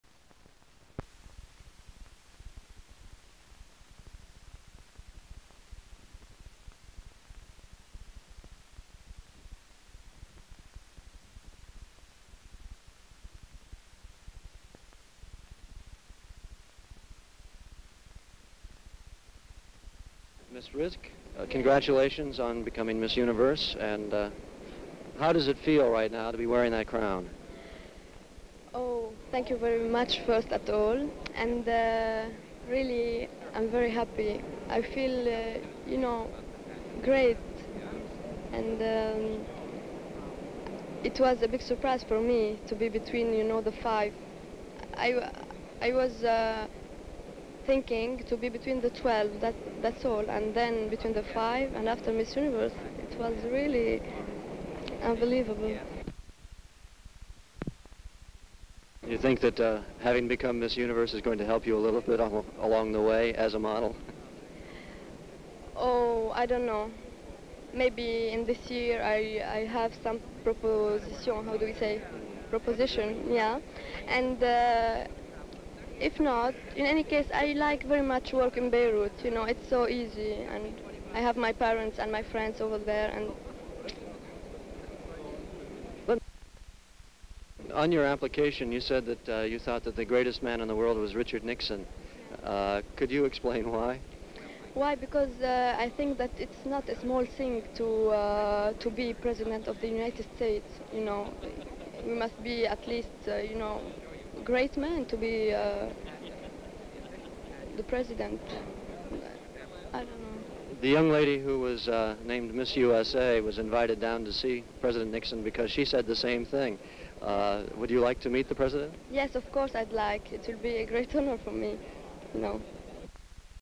SYND 29-7-71 INTERVIEW WITH MISS UNIVERSE
(28 Jul 1971) The 18 year old Lebanese contestant Georgina Rizk visits New York as Miss Universe winner.